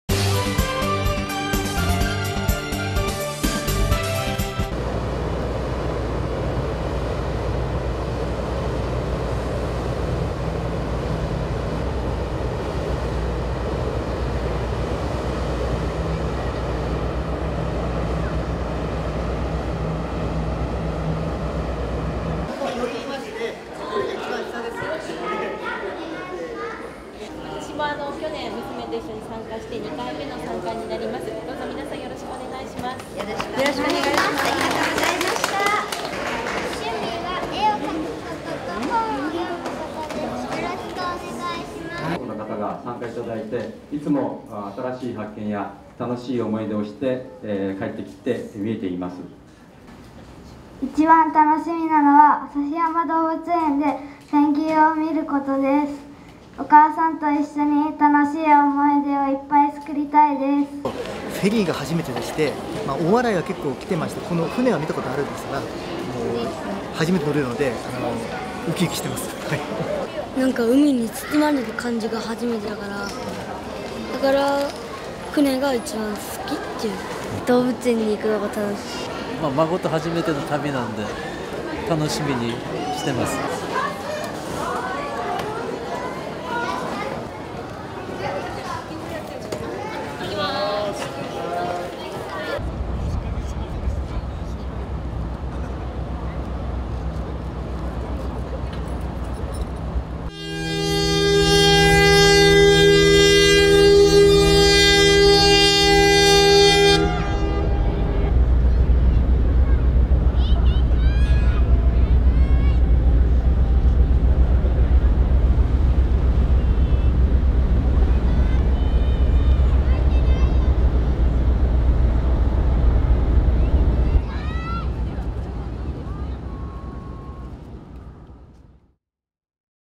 親子ふれあいクルーズ ９１組が北海道へ出発 大洗
北海道を巡る旅を通して親子の絆を強める「親子ふれあいクルー ズｉｎ北海道」（茨城新聞社、下野新聞社、上毛新聞社主催、日立ポートサービス企画・実施）の第１班は27日夕、大洗町の茨城港大洗港区を出発した。
　茨城・栃木・群馬３県から小学生の親子計91組１８２人が参加し、４泊５日の旅を楽しむ。一行はおよそ19時間の船旅を経て、28日昼に北海道の苫小牧港に到着する予定。札幌市の定山渓温泉などに２泊し、旭川市の旭山動物園や富良野町のラベンダー畑、千歳市の箱根牧場といった名所を巡り、夏の北海道を満喫する。
　出航前のセレモニーでは各県１人ずつあいさつ。行方市玉造甲、市立玉造小４年、関口夢来さん（９）は「旭山動物園でペンギンを見るのが楽しみ。お母さんと楽しい思い出をつくりたいです」と元気いっぱいに話した。
　一行は、一夏の思い出づくりに期待を膨らませ、大型フェリーの「さんふらわあさっぽろ」に乗り込んだ。